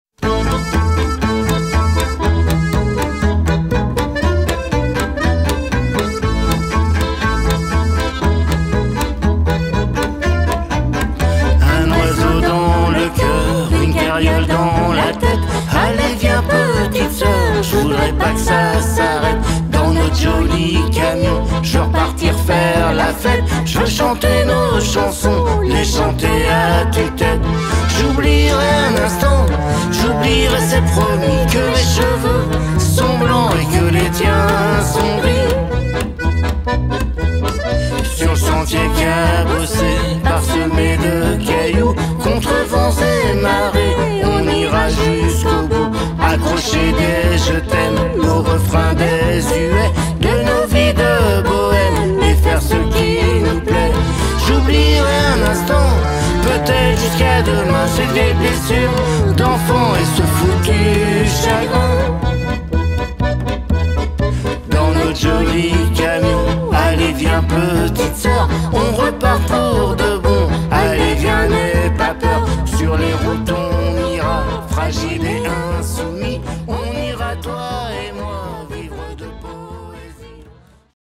Sortie d'albums
Le 2 avril à 15h30 sous le beau chapiteau 🎪 du Centre Musical International de Roussigny (72320 Saint Ulphace)
Allez tu viens?!! 🙃🥳